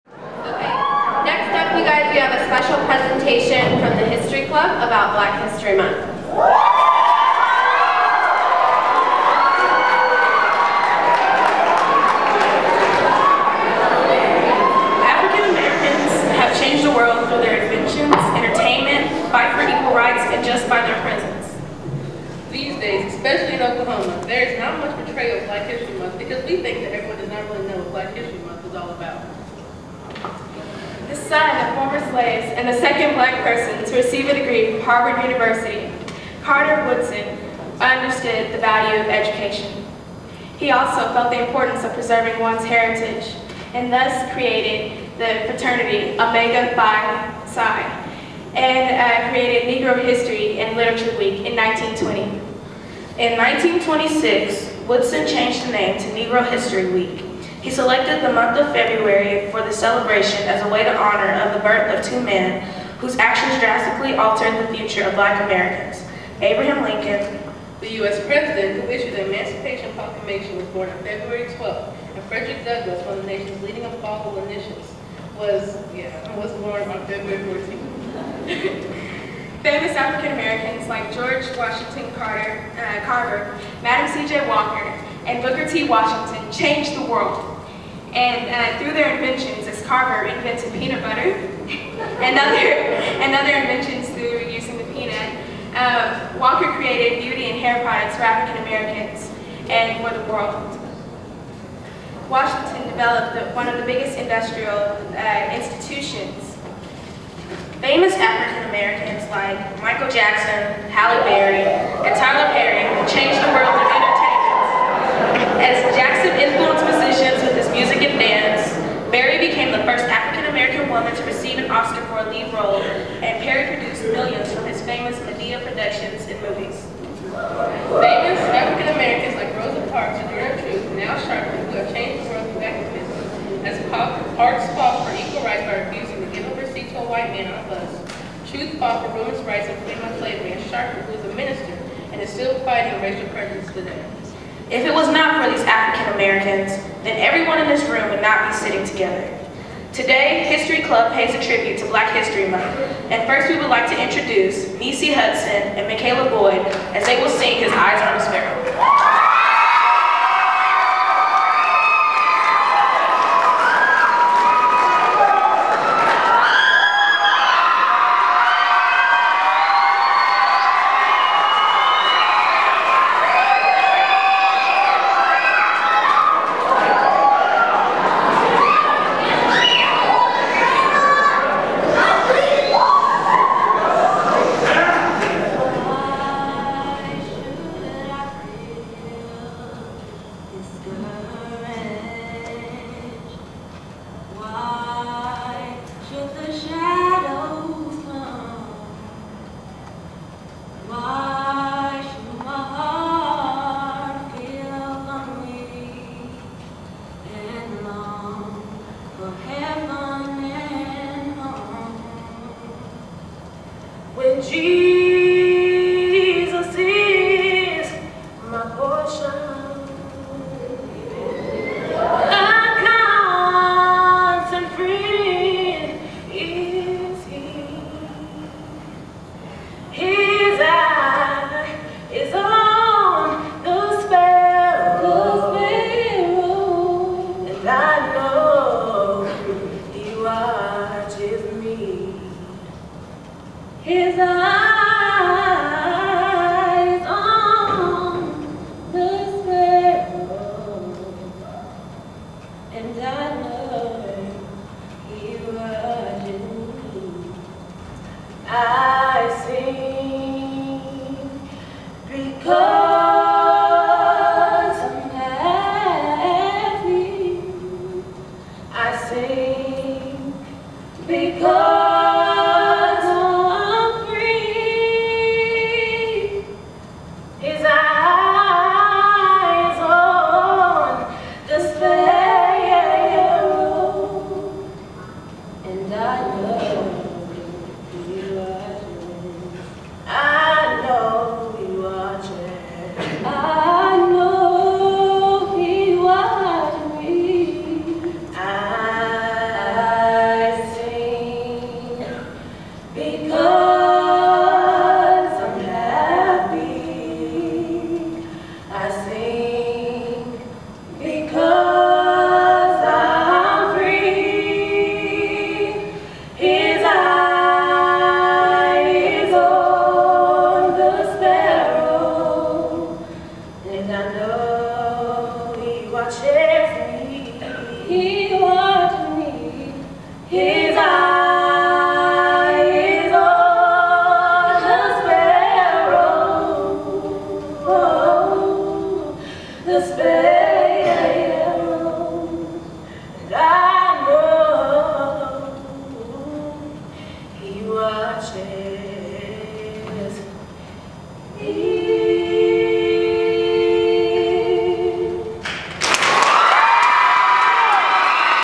Harding Charter Prep Presents Black History Month, Part 1
Students at Harding Charter Prep in OKC give their presentation for Black History Month